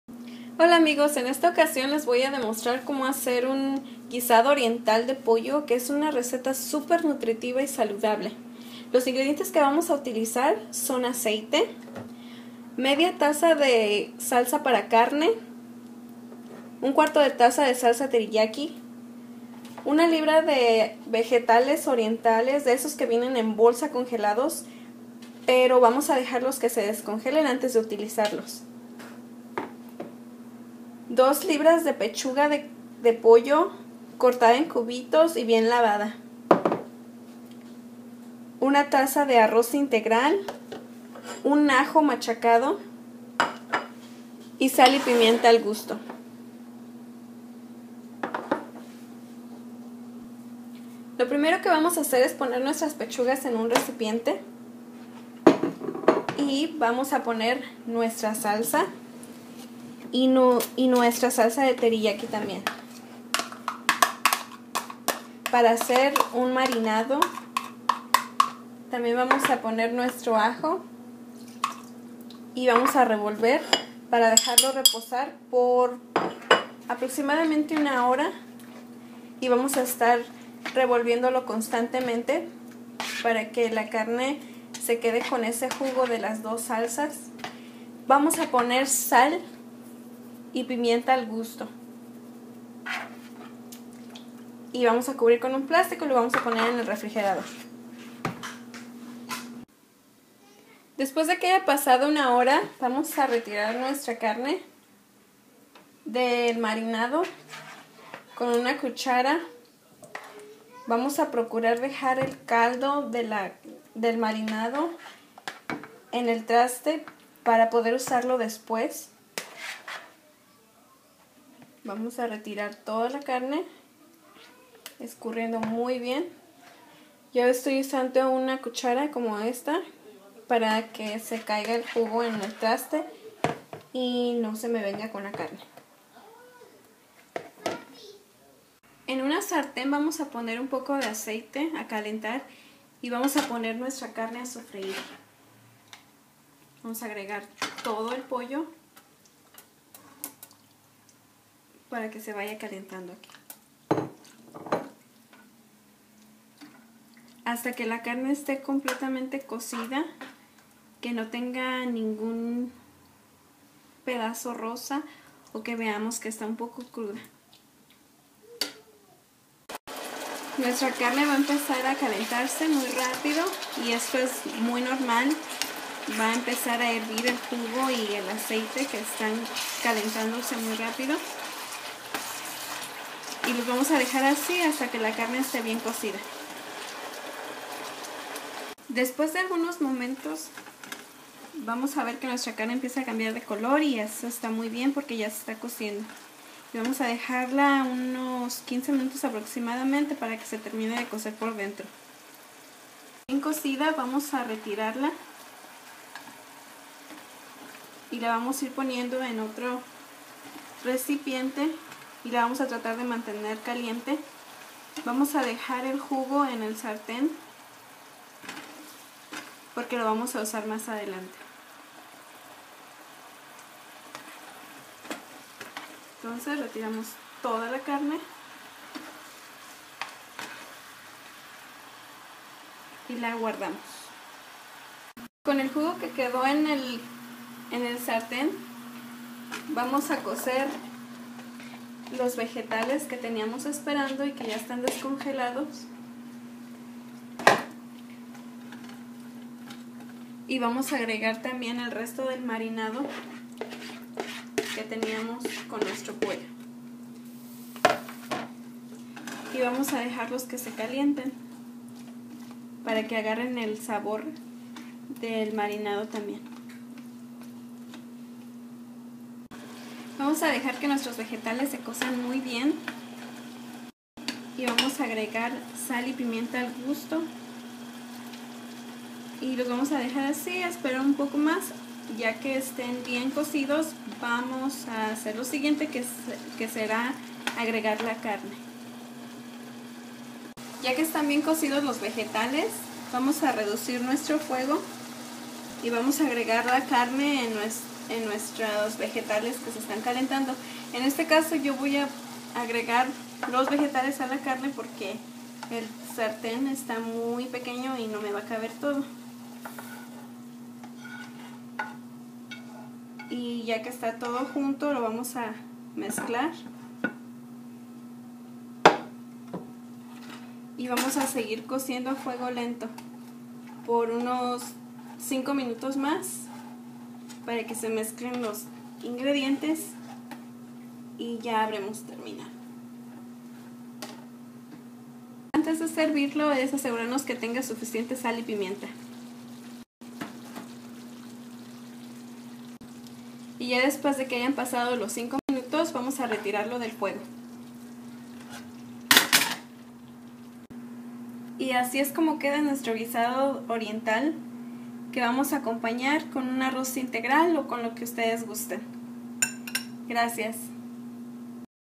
Receta oriental de pollo
Este tutorial te muestra paso a paso cómo hacer un guisado oriental de pollo. Es una receta sencilla, deliciosa y muy nutritiva que puedes preparar para el almuerzo o la cena. Los ingredientes que necesitarás son: aceite, salsa para carne, salsa teriyaki, vegetales congelados, pechugas de pollo, arroz integral, ajo, sal y pimienta.

¡Descárgala gratis y gana dinero creando tus propios vídeos!